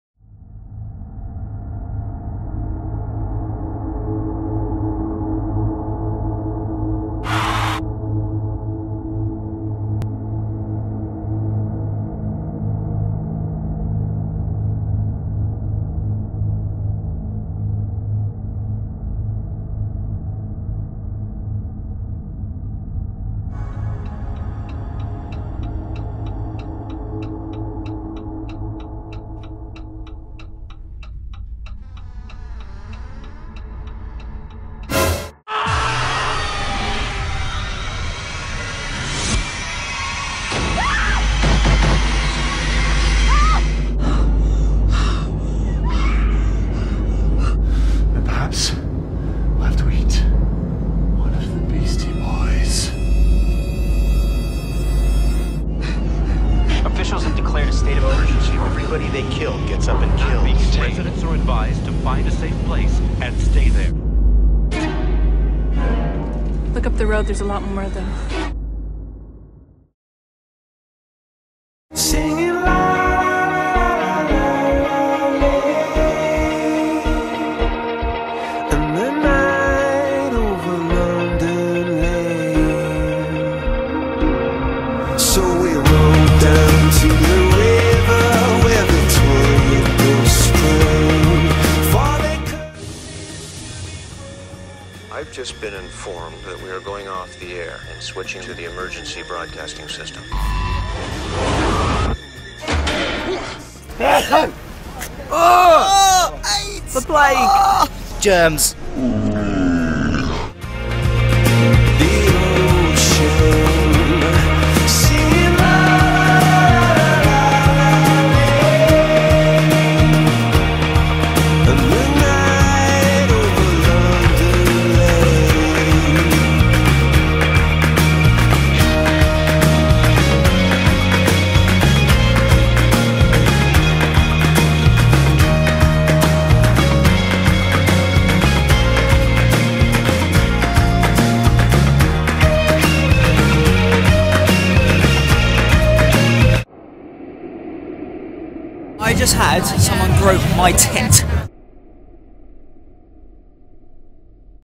Dawn of the Dead - The Midnight Beast Spoof Trailer (HD)
So this is your Halloween gift...I suppose...

It was really hard to edit this one cos iMovie was being slow.
Ah well, enjoy! :D